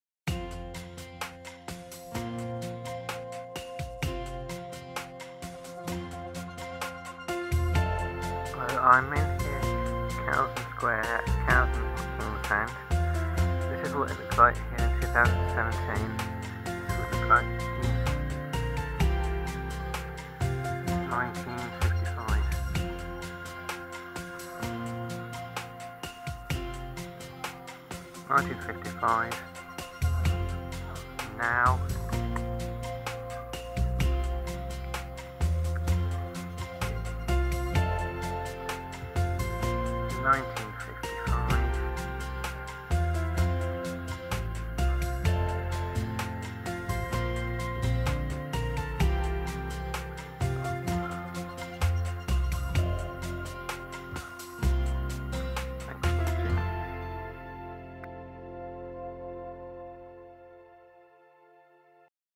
Cawsand - Now & Then
This is Cawsand in Cornwall - What it looks like now & then!!!